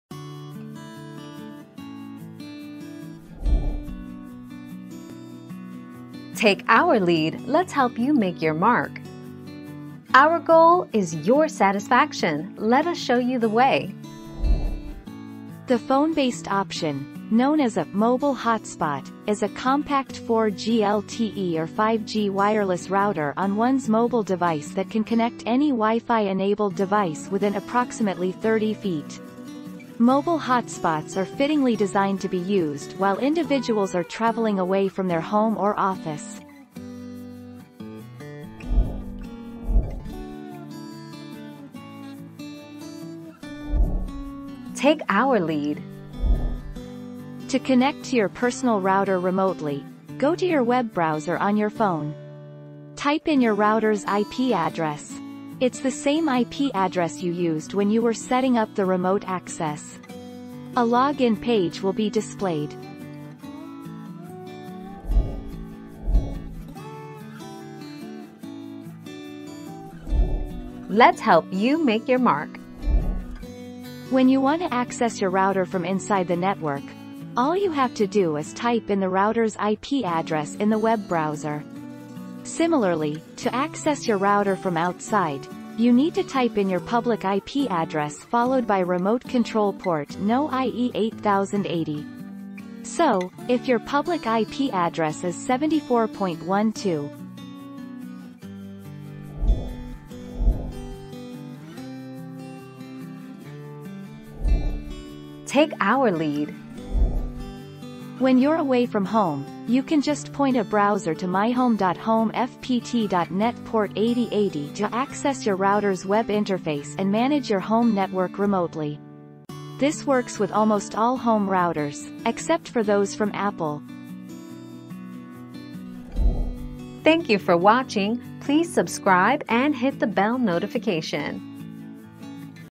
Can I connect to my WiFi away from home?
00:00 - Can I connect to my WiFi away from home?
00:40 - How can I access my home router from anywhere?
01:06 - How can I access my router from anywhere?
01:41 - How do I connect to my home network from anywhere?

Laura S. Harris (2021, September 25.) Can I connect to my WiFi away from home?
    AskAbout.video/articles/Can-I-connect-to-my-WiFi-away-from-home-264199